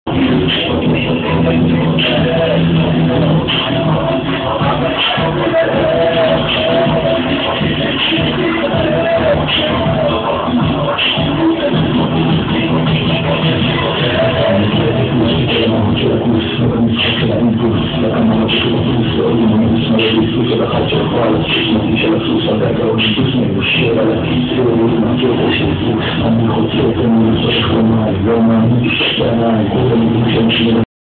Dj Niro in Ultrasound ,Israel
Nir Lazimi AKA Dj Niro in The Ultrasound Club ,From Israel.